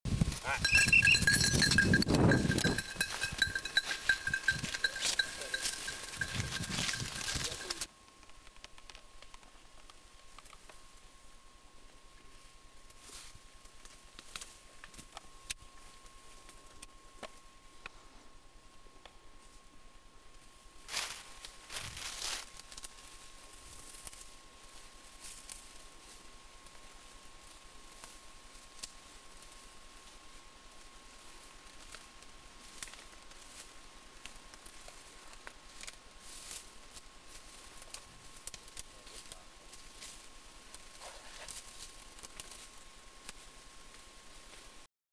Blue Streak
Jerry Kolter and Blue Streak on breakaway and then on point